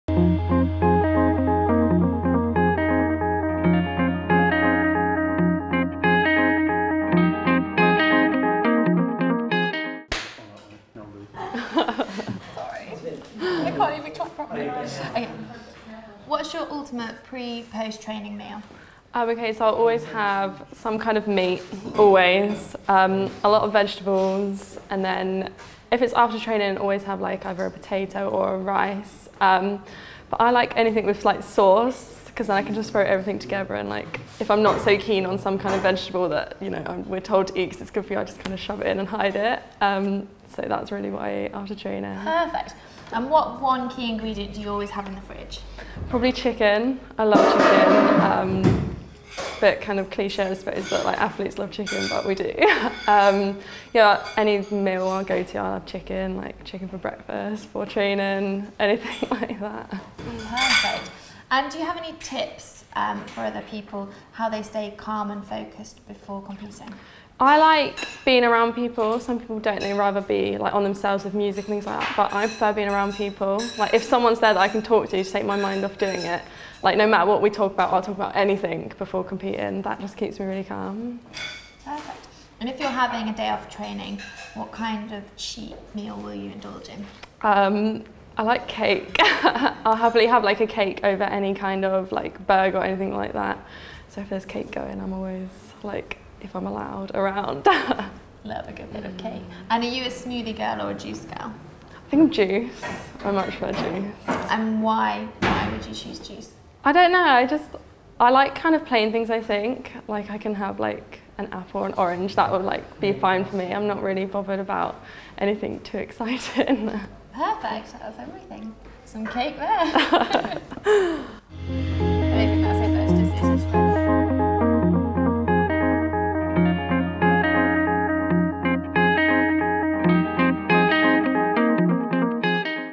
What does an elite, young British sprinter keep in the cupboards at home?
We sat with elite British sprinter Sophie Papps and asked her some quick-fire questions on nutrition for Thrive Magazine...